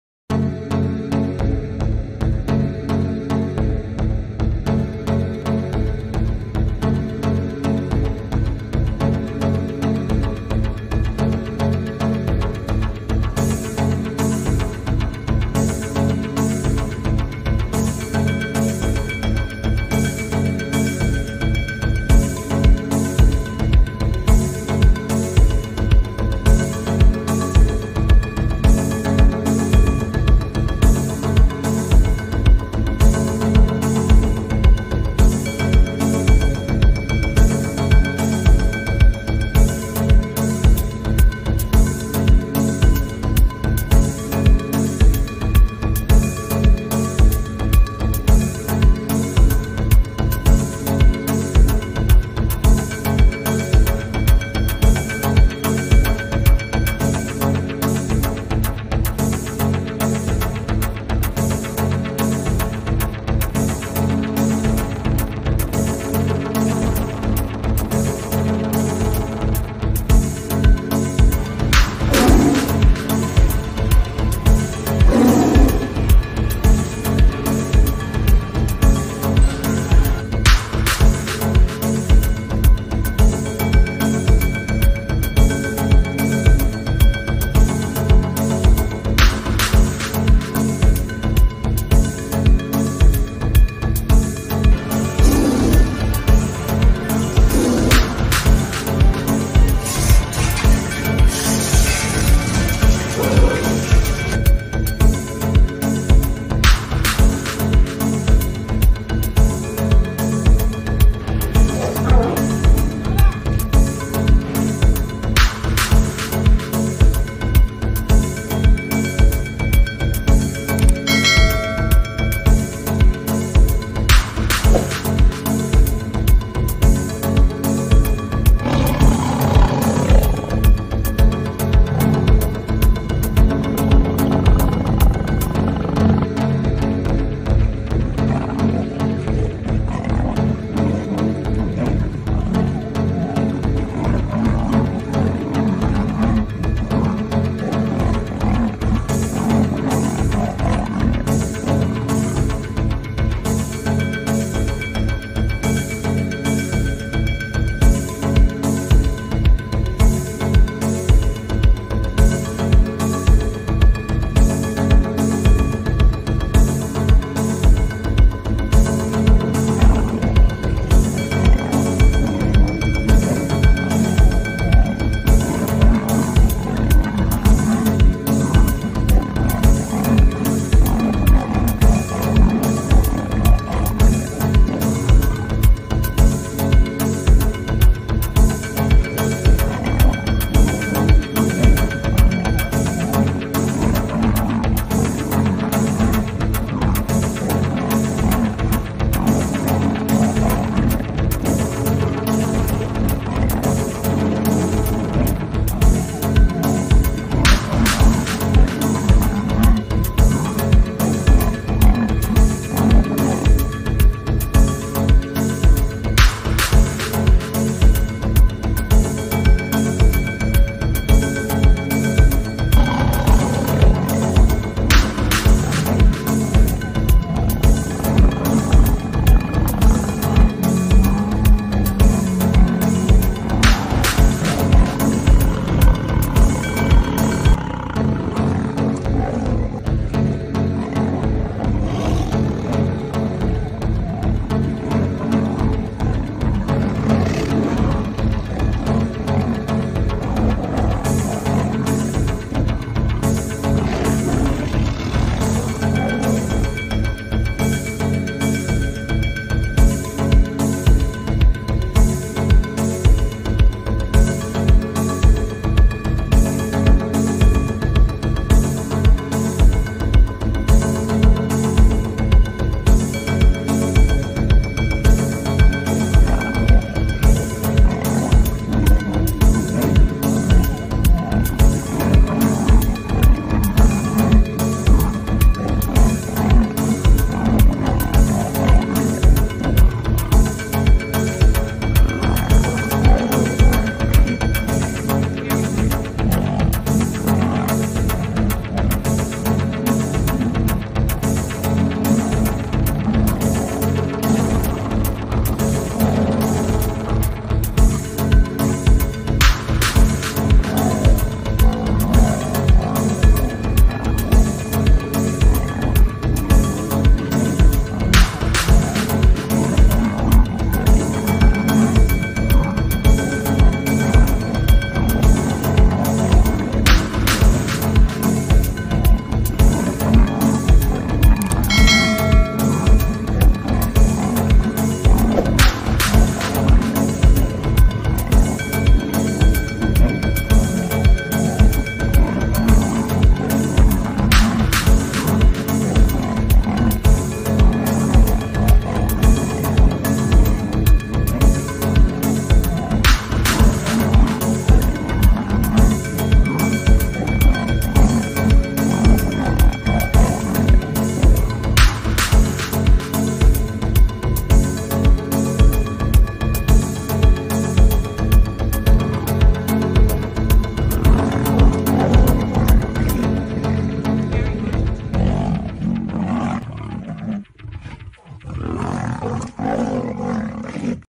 Pertarungan singa vs harimau.
pertarungan 2 raja rimba sama sama mempunyai kekuatan keduanya
pemangsa nomer satu di alam liar.

vidio ini hanya untuk hiburan.